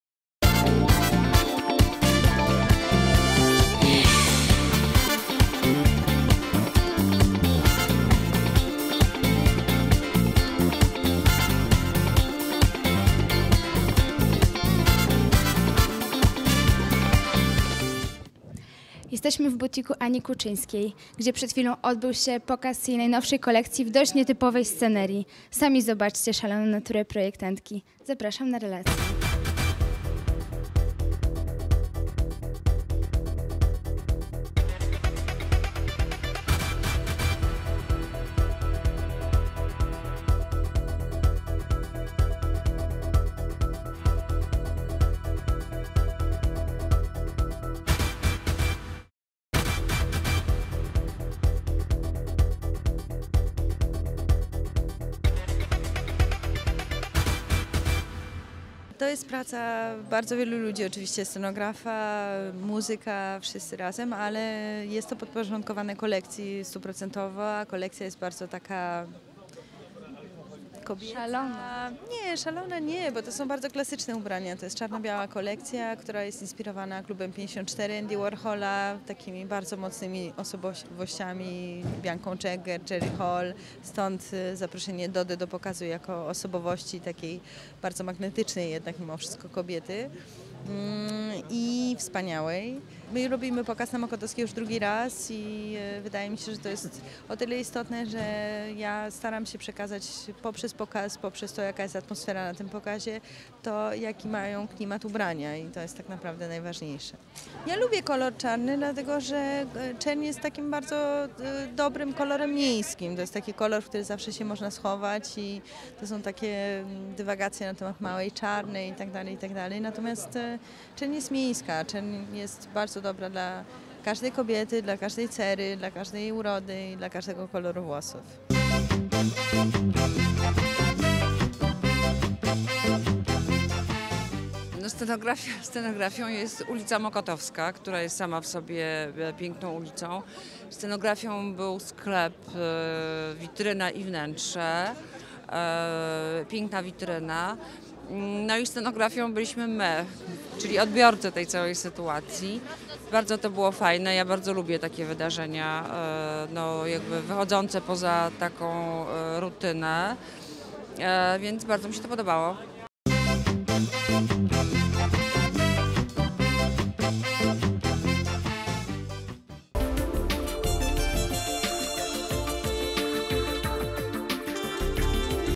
Ania Kuczyńska - pokaz kolekcji PIÑATA wiosna/lato 2012
Niedawno byliśmy świadkami pokazu kolekcji wiosna/lato 2012 Ani Kuczyńskiej. Projektantka, zwana królową minimalizmu, po raz kolejny pokazała ubrania o prostych krojach, w których niezmiennie dominowała czerń i biel.

Scenografią była ulica Mokotowska, a modelki prezentowały stroje tańcząc w witrynie butiku Kuczyńskiej. Pokaz wywarł ogromne wrażenie na przybyłych gościach. Na koniec w roli modelki wystąpiła Doda. Autorka pomimo prostych krojów przemyciła odrobinę szaleństwa, które dostrzegliśmy w dość niezwykłej choreografii.

D.Pacańska